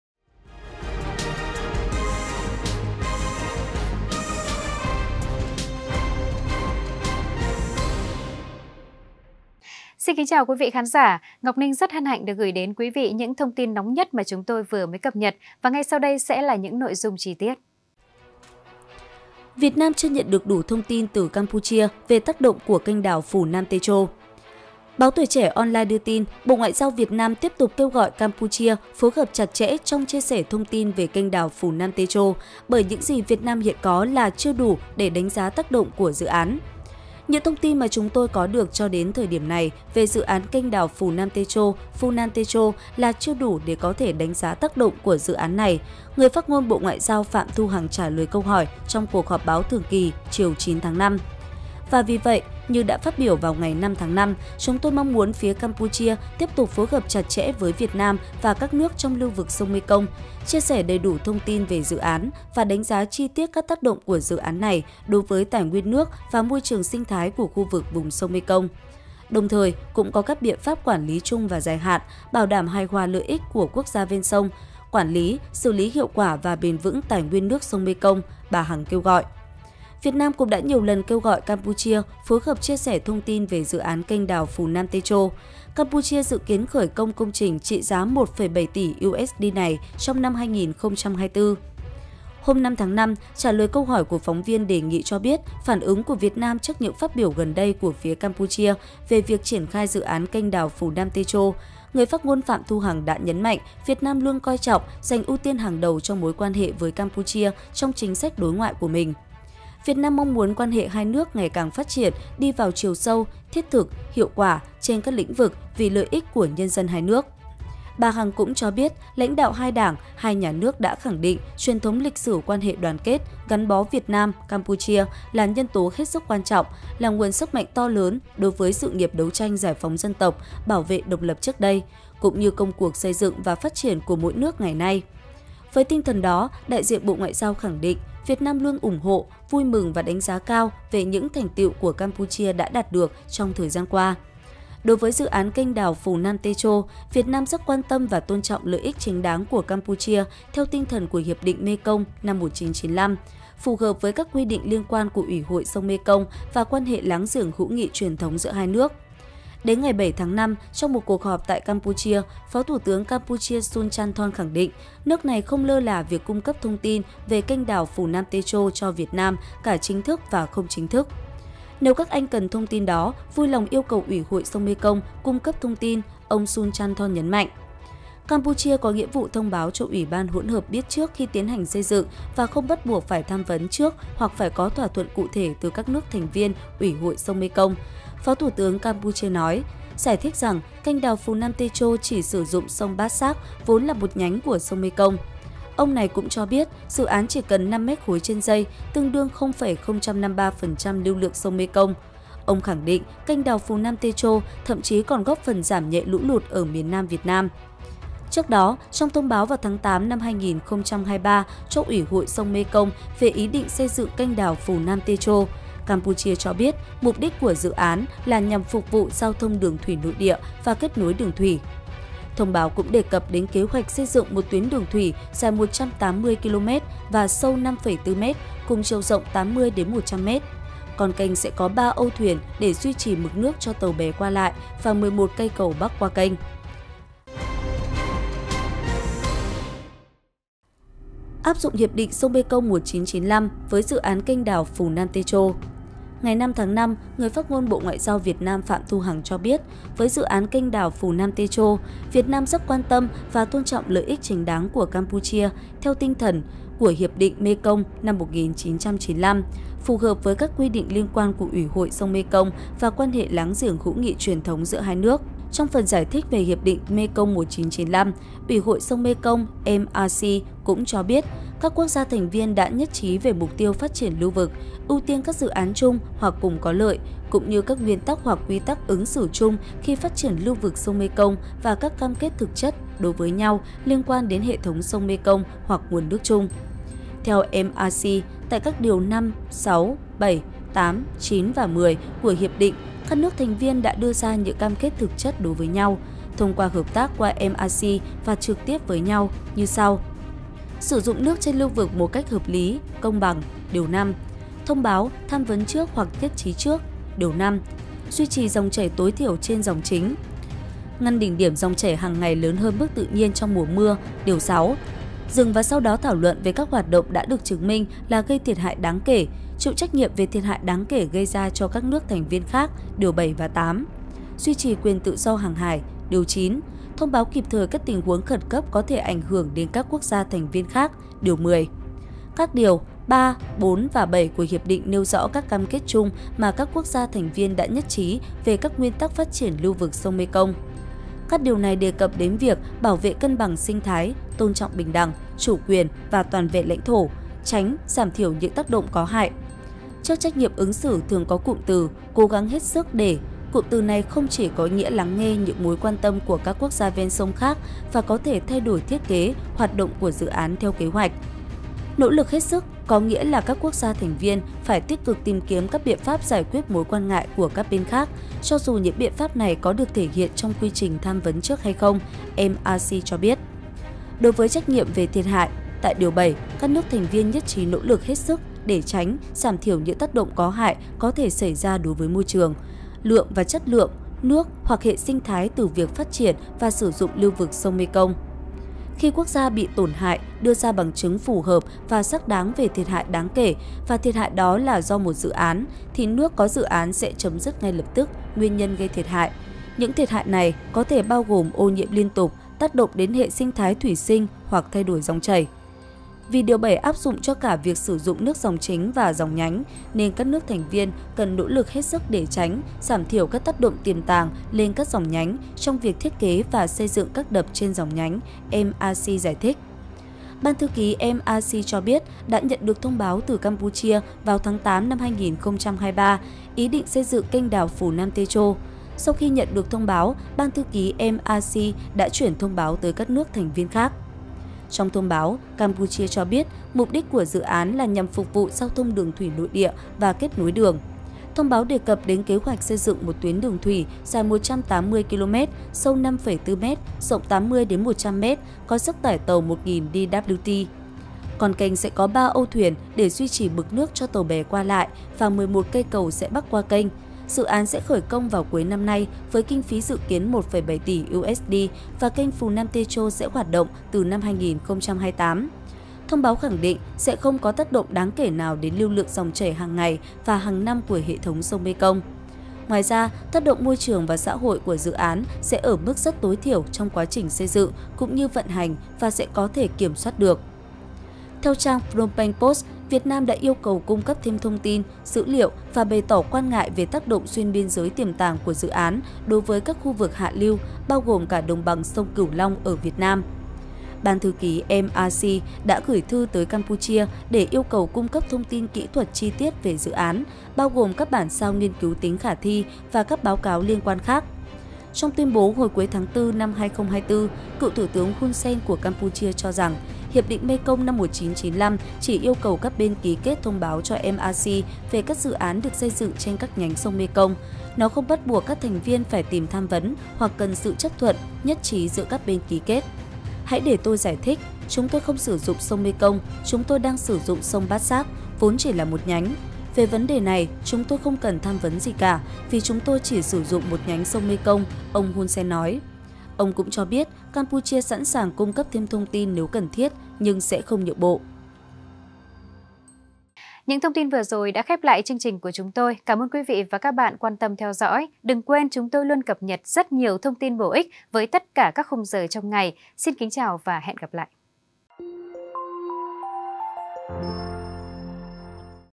Việt Nam chưa nhận được đủ thông tin từ Campuchia về dự án Funan Techo | CafeLand
CafeLand | Báo Tuổi trẻ đưa tin, Bộ Ngoại giao Việt Nam tiếp tục kêu gọi Campuchia phối hợp chặt chẽ trong chia sẻ thông tin về kênh đào Phù Nam Techo, bởi những gì Việt Nam hiện có là chưa đủ để đánh giá tác động của dự án. "Những thông tin mà chúng tôi có được cho đến thời điểm này về dự án kênh đào Phù Nam Techo (Funan Techo) là chưa đủ để có thể đánh giá tác động của dự án này", người phát ngôn Bộ Ngoại giao Phạm Thu Hằng trả lời câu hỏi trong cuộc họp báo thường kỳ chiều 9-5.
"Và vì vậy, như đã phát biểu vào ngày 5-5, chúng tôi mong muốn phía Campuchia tiếp tục phối hợp chặt chẽ với Việt Nam và các nước trong lưu vực sông Mekong, chia sẻ đầy đủ thông tin về dự án, và đánh giá chi tiết các tác động của dự án này đối với tài nguyên nước và môi trường sinh thái của khu vực vùng sông Mekong.
Đồng thời cũng có các biện pháp quản lý trung và dài hạn, bảo đảm hài hòa lợi ích của quốc gia ven sông, quản lý, xử lý hiệu quả và bền vững tài nguyên nước sông Mekong", bà Hằng kêu gọi. Việt Nam cũng đã nhiều lần kêu gọi Campuchia phối hợp chia sẻ thông tin về dự án kênh đào Phù Nam Techo. Campuchia dự kiến khởi công công trình trị giá 1,7 tỉ USD này trong năm 2024.